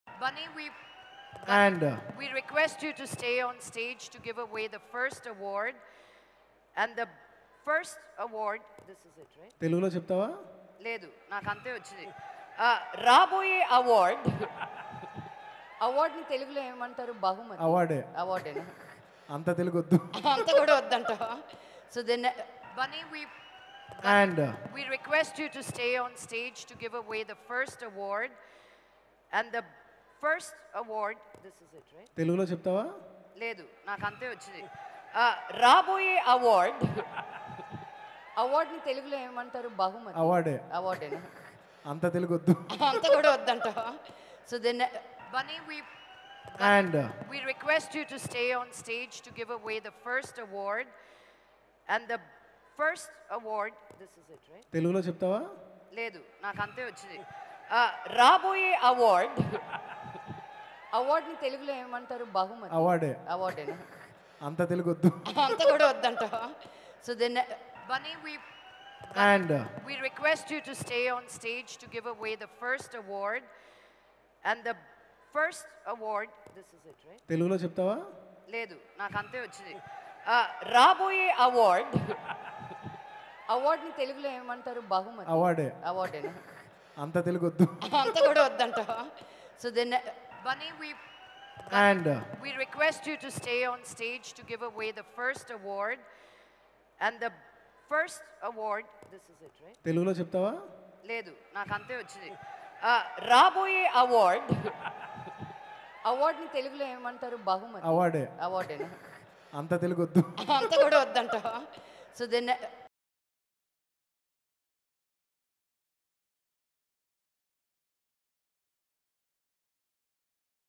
Allu Arjun Satirical Comments On Manchu Laxmi At AHA 2.0 || Tollywod Update || TV39 Telugu
Watch:-Allu Arjun Satirical Comments On Manchu Laxmi At AHA 2.0 || Tollywod Update || TV39 Telugu ||